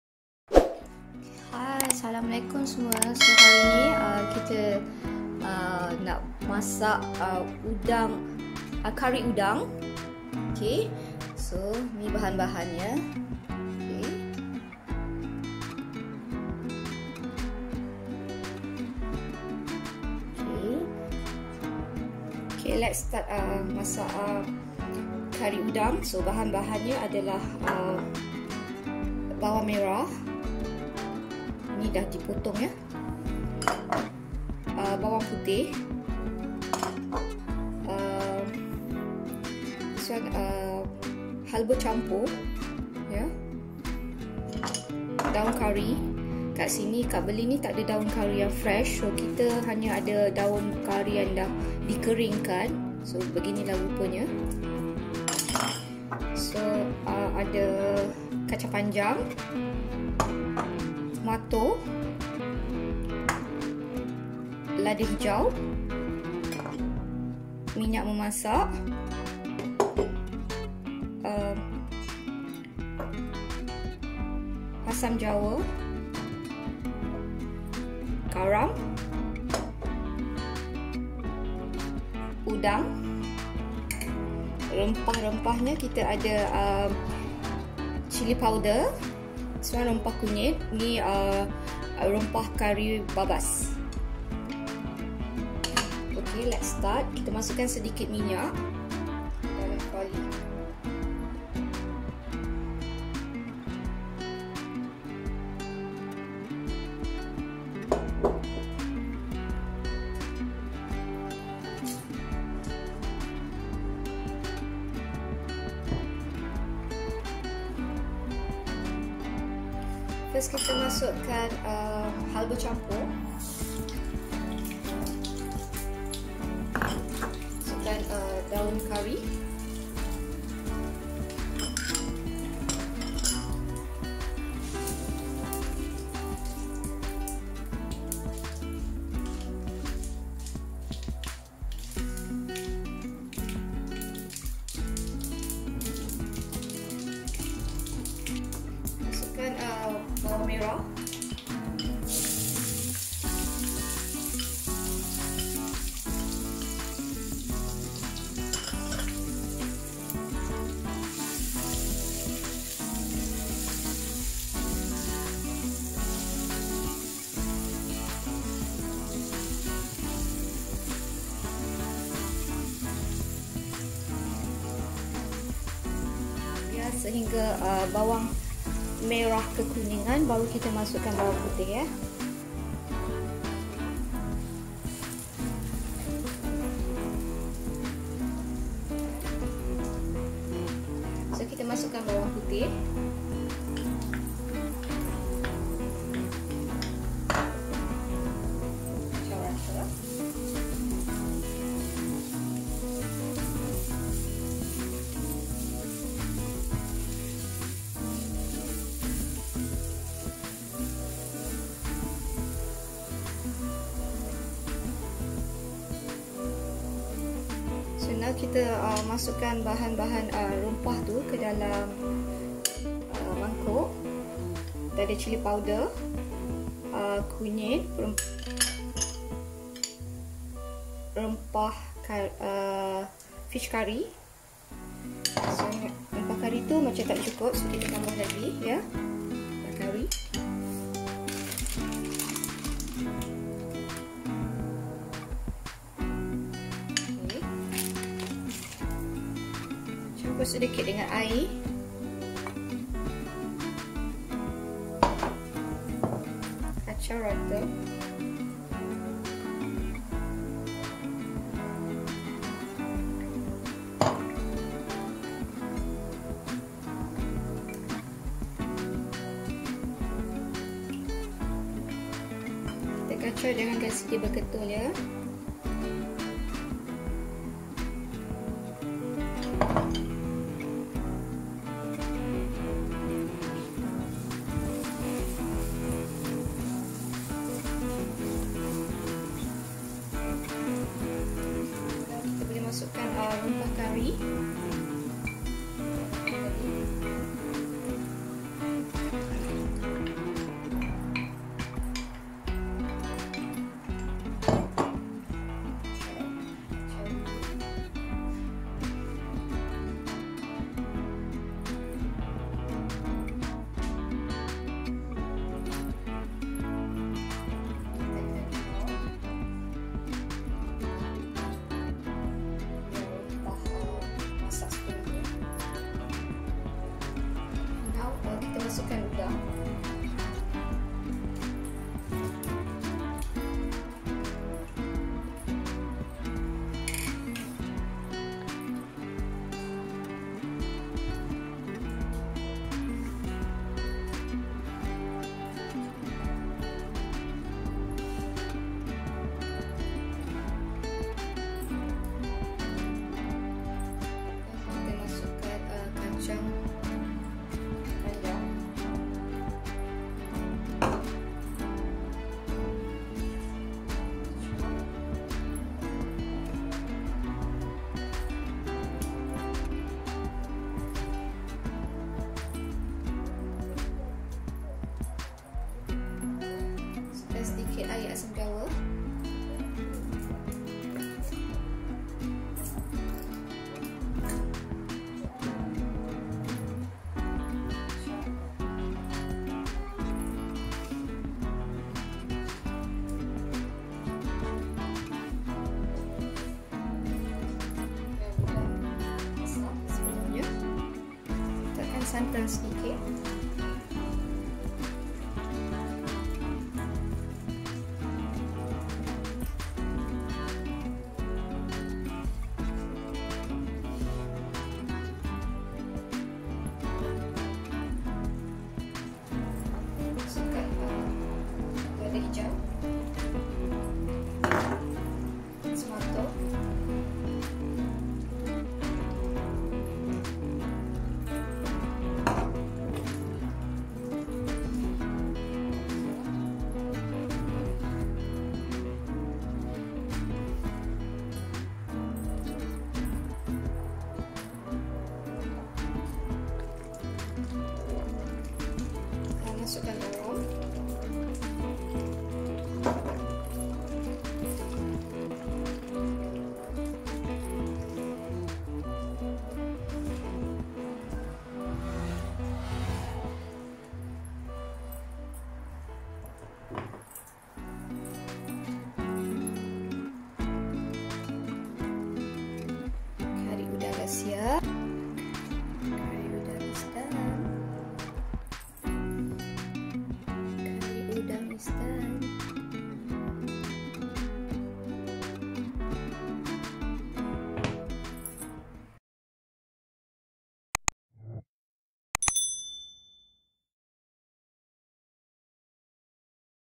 'KARI UDANG' Tak KAW tapi BERTAMBAH² Makan. Made in Berlin!
Menu hari ini adalah 'Kari Udang made in Berlin'. Nak kata tak Kaw2 tapi memang sedap dan sedap lagi dengan bahan2 yang limited di sini Germany. Harap2 dengan upload video ini, anda boleh la merasa Kari Udang ni :)
Happy Cooking semua...

Bahan2 yang perlu:

1. Bawang Besar 1 biji
2. Bawang Putih 4 biji
3. Halba Campur
4. Daun Kari
5. Kacang Panjang
6. Tomato
7. Lada Hijau
8. Asam Jawa
9. Garam
10. Udang 250g
11. Rempah Kunyit 1 sudu kecil
12. Cili Powder 1 sudu besar
13. Rempah Kari Ikan 2 sudu besar
14. Santan

### English ###

Today Menu is 'Curry Prawn made in Berlin'. So to say is not Power but yet is so delicious and satisfied especially with limited ingredients here in Germany. Hope with this upload everyone can taste Curry Prawn :)
Happy Cooking Everyone...

Ingredients needed:

1. Onion 1 pcs
2. Garlic 4 pcs
3. Mixed Fenugreek
4. Curry Leaves
5. Long Beans
6. Tomato
7. Green Chili
8. Tamarind
9. Salt
10. Prawn 250g
11. Tumeric Powder 1 tea spoon
12. Cili Powder 1 spoon
13. Curry Fish Powder 2 spoon
14. Coconut Milk